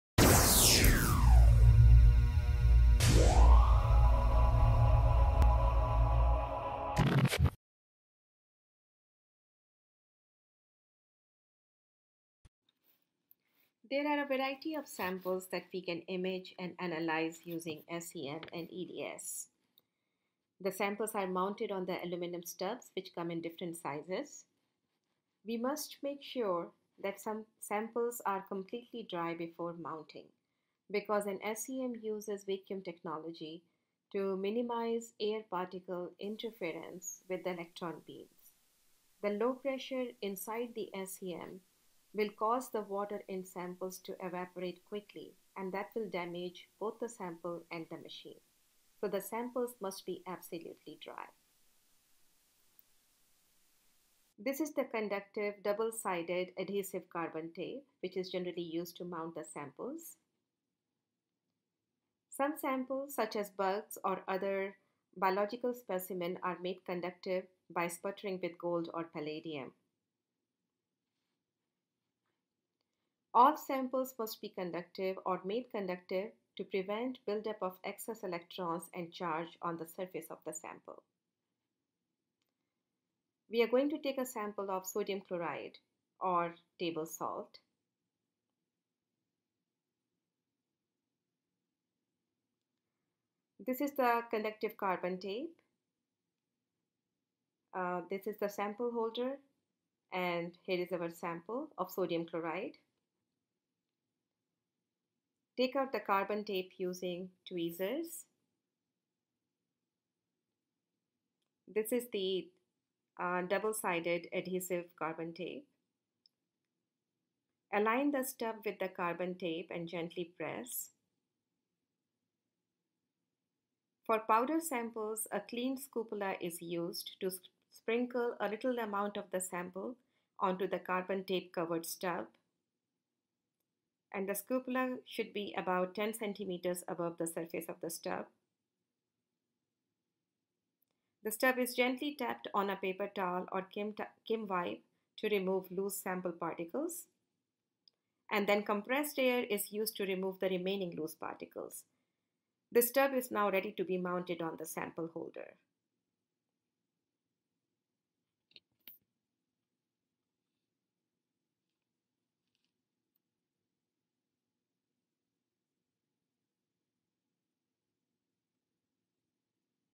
Phenom SEM Overview - Part 2: Placing a Sample on a Stub for Analysis (NVCC 02/13)
Demonstration of how to place a sample on a stub for analysis using the SEM.